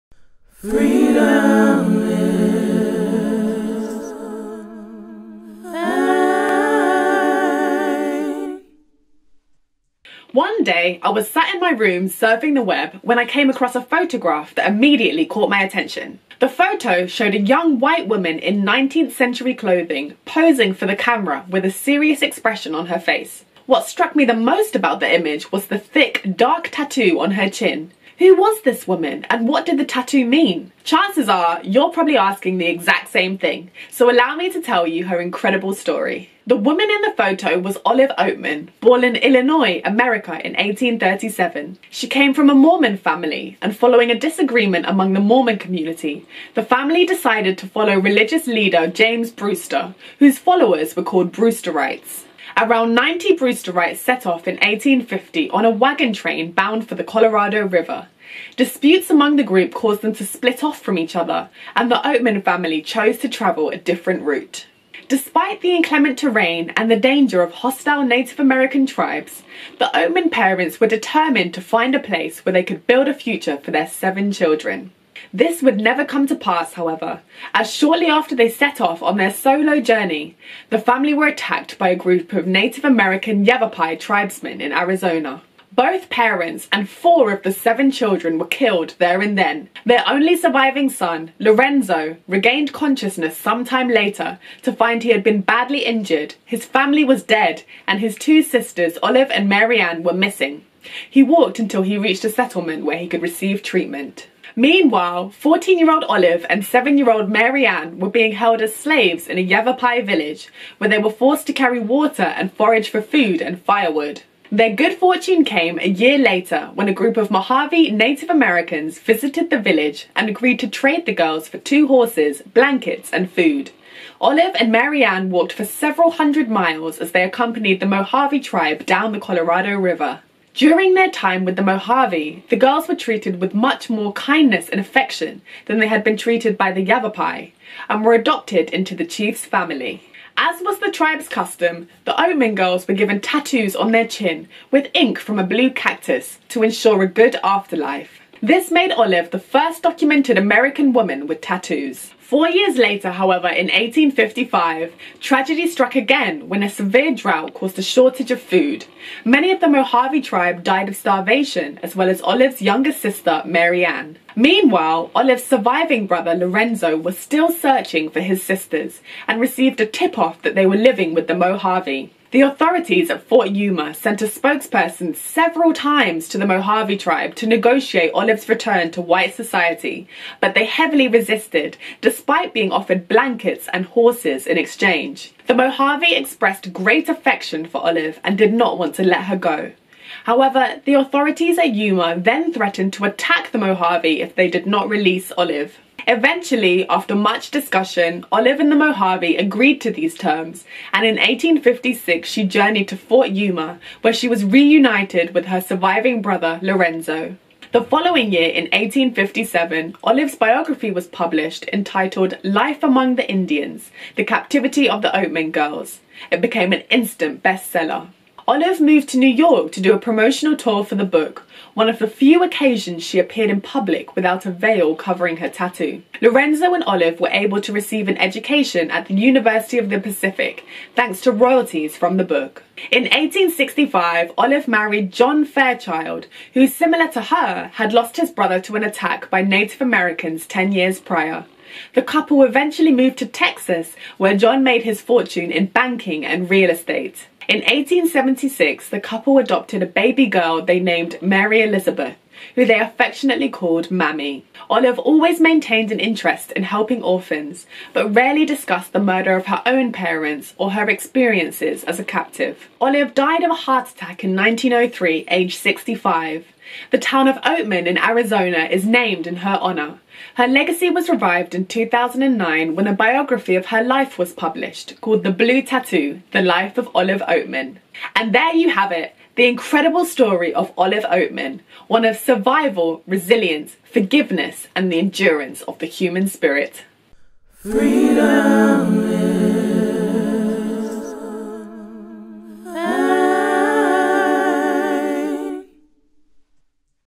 The Girl With The Face Tattoo
Ever heard the incredible story of Olive Oatman? Who was this woman and what did her tattoo mean? This is a tale of kidnapping, slavery, murder and identity. Watch to find out!

Illustration by: Jimena Isabel Merchán & Ana Ferreras. Find them on Instagram at: @jimenaimm and @annilustra

Facebook: Freedom Is Mine Official